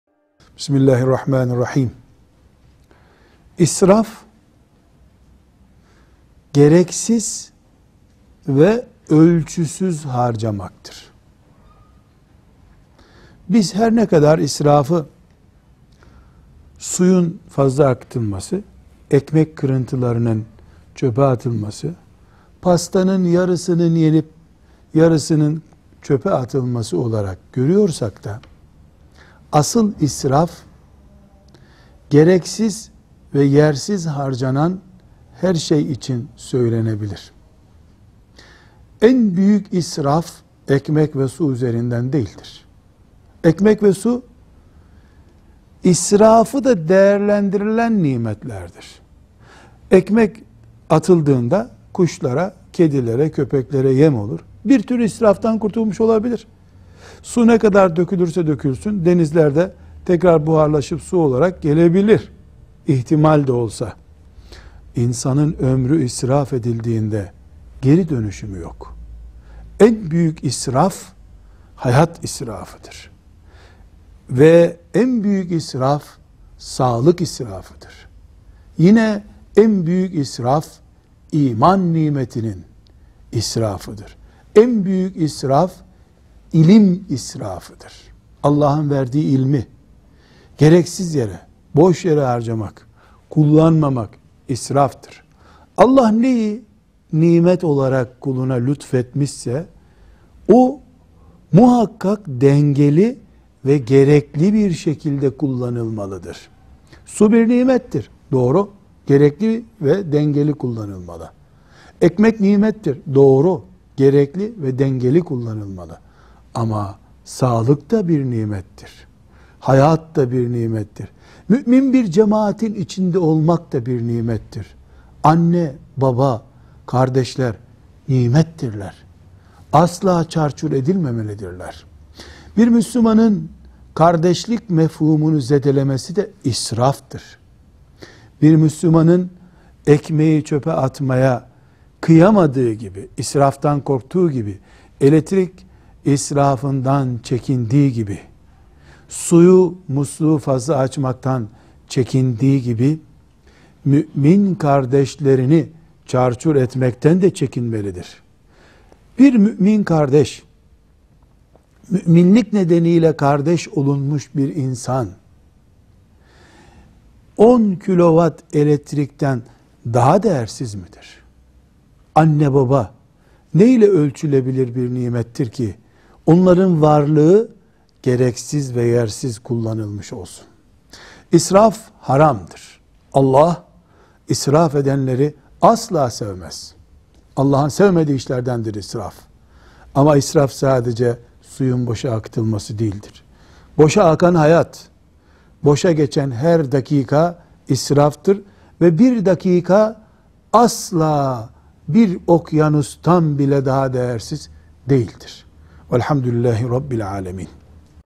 Israf nedir - Nureddin YILDIZ
İşaret Dili Anlatımlı. Hayirlara vesile olur insaALLAH. Nureddin Yildiz Hocamizin bu konudaki görüslerini lütfen izleyin.